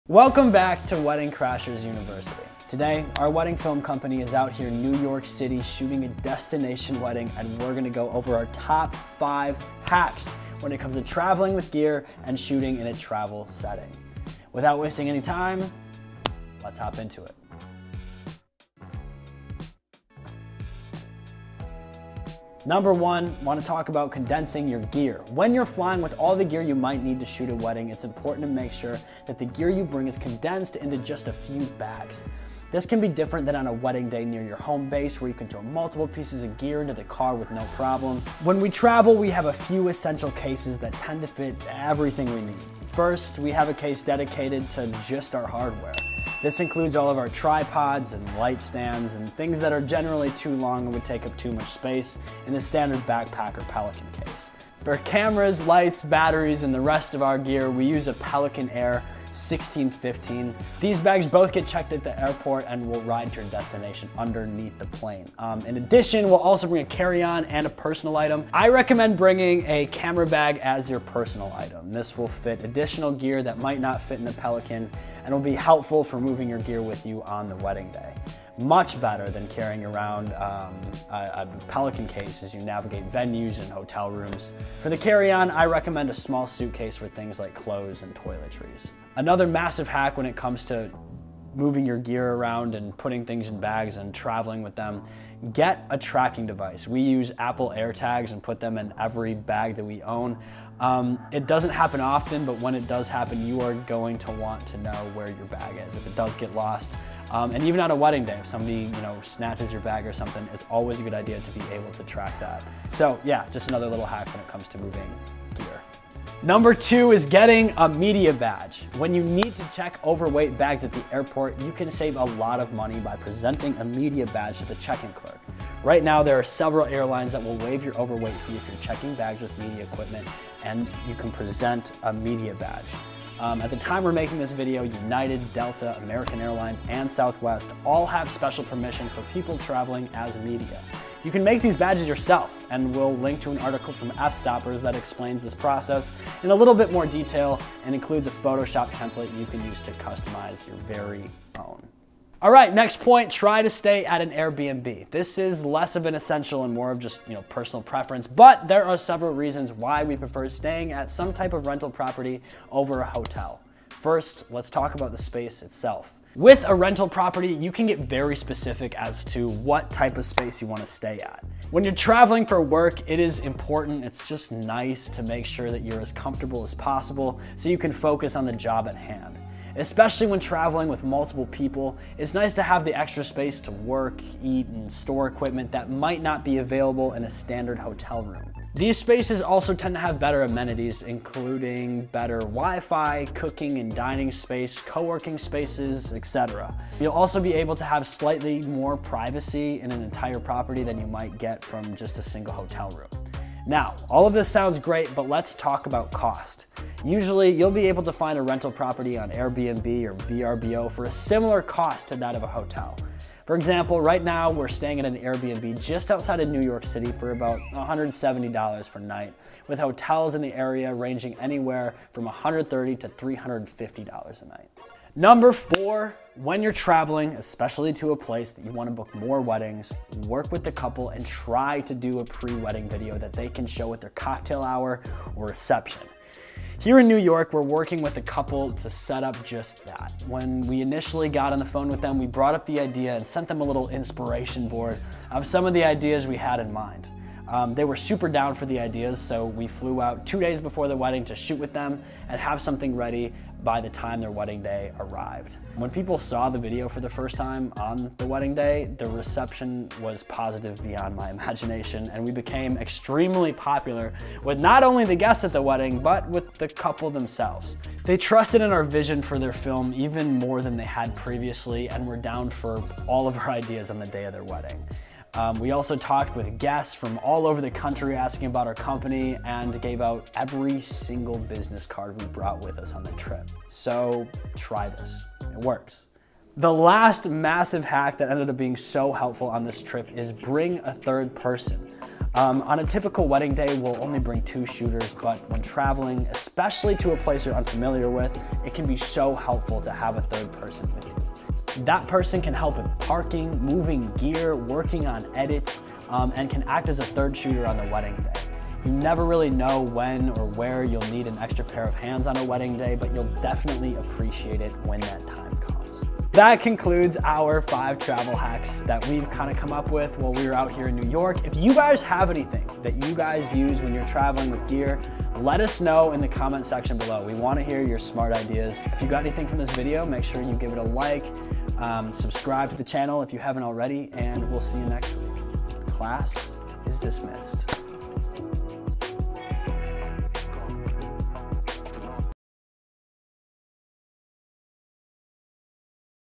Traveling the World, One Wedding at a Time: Travel Tips from a Destination Wedding Filmmaker
Discover the secrets of packing smartly, optimizing your gear setup, and maintaining the highest level of professionalism while on the road. We provide expert tips on staying organized, protecting your equipment, and adapting to different environments to deliver exceptional results no matter where you are.

Godox CB-01 Wheeled Light Stand and Tripod Carrying Bag

Pelican Air 1615 Case with Trekpack Dividers

Apple AirTag